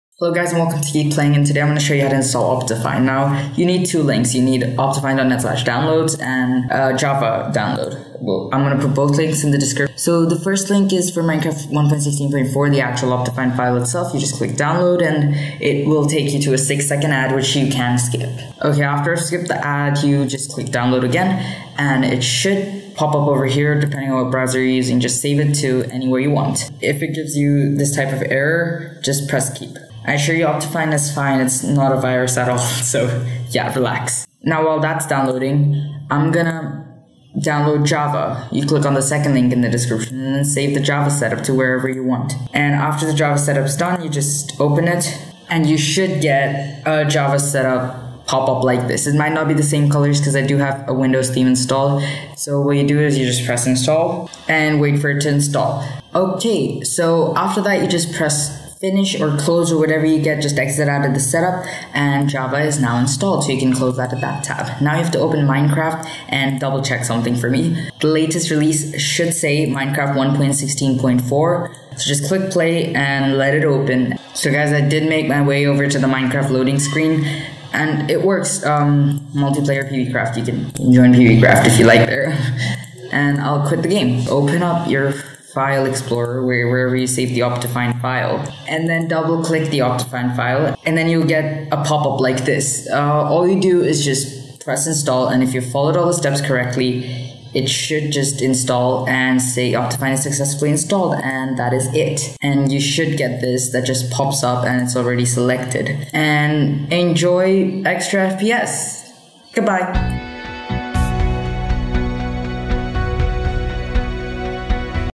How To Install OptiFine 1.16.4 Easy and Fast 2020!
Hey guys, today I have a quick video showing you how to install OptiFine with no BS. Here are the 2 links you need: